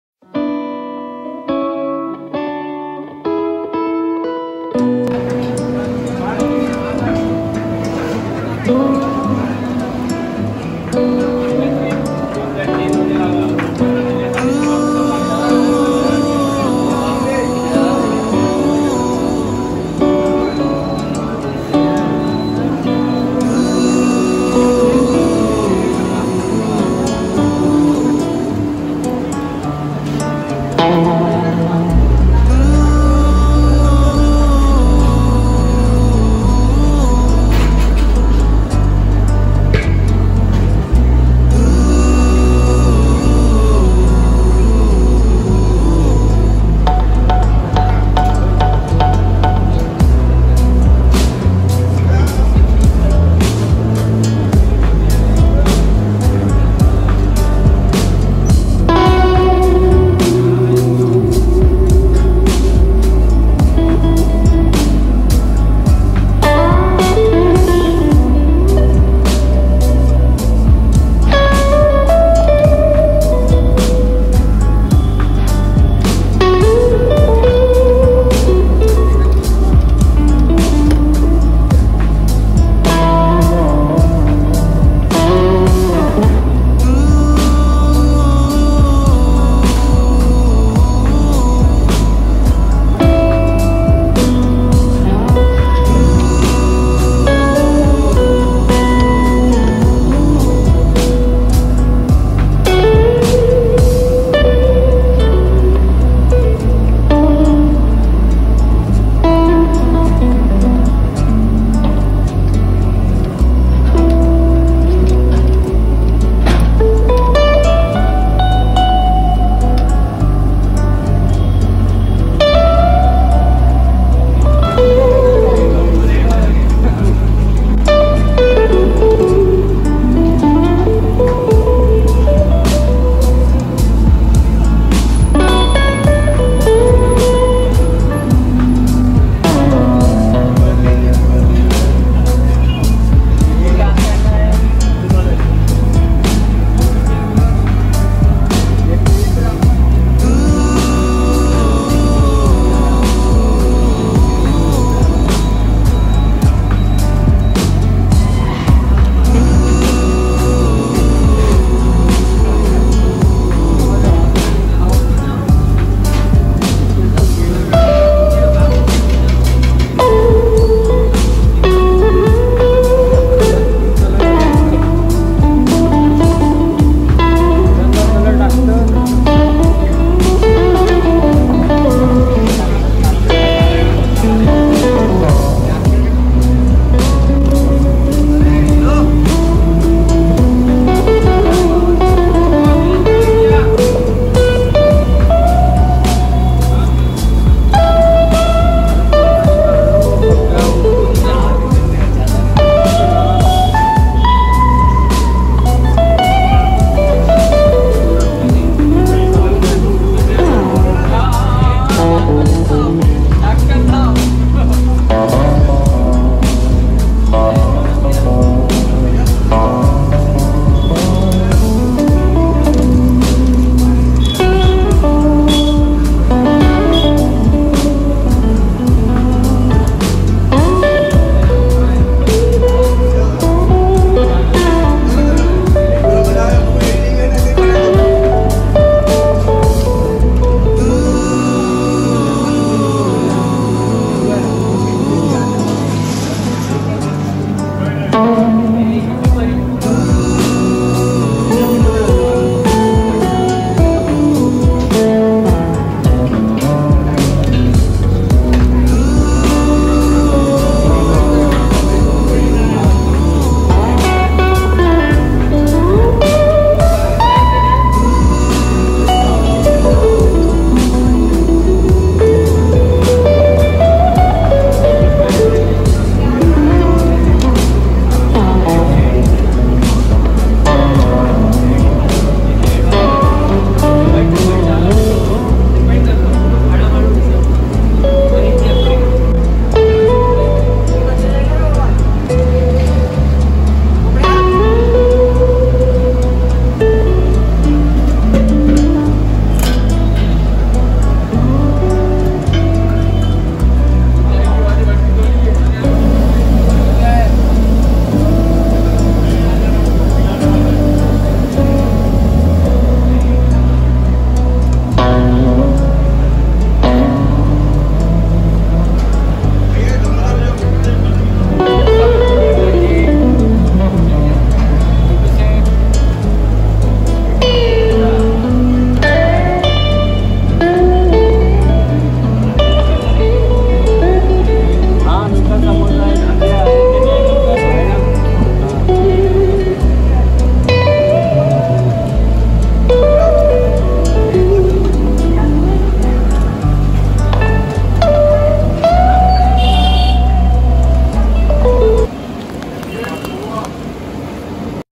Sindhi Gheeyar|Holi special sweet| सिंधी घीयर|How to make sindhi gheeyar|indian street food
Happy Holi to all the fam❤️
I am sharing this absolutely amazing sweet dish Gheeyar/Ghevar. This Gheeyar is famous in Sindhi  community. They started their Holi morning with this authentic Sindhi Gheeyar.
📍pankaj kadhiwale, jaripatka Nagpur